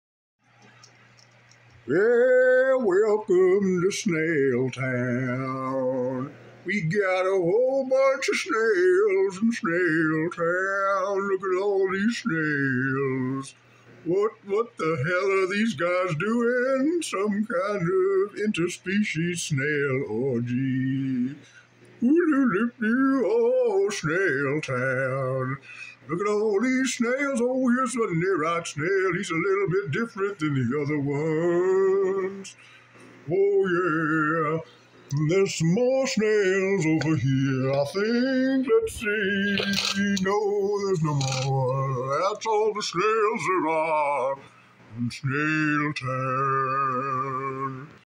The Snail Town 2023 Musical Extravaganza
Let the beautiful singing be your guide on this tour of SNAIL TOWN. Get your snail on, you son of a bitch.